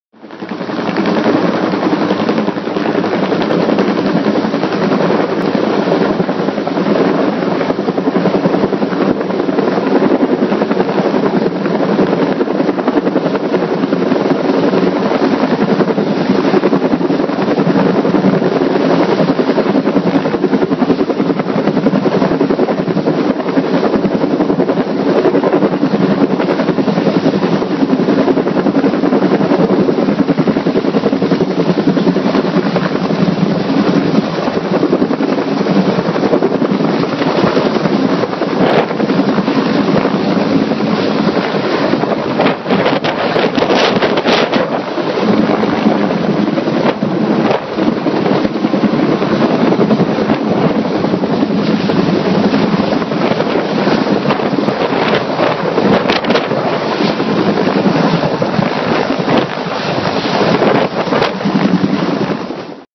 Another Day On The Job For Jason Clause
Helicopter sticking a Power Pole in less than a minute.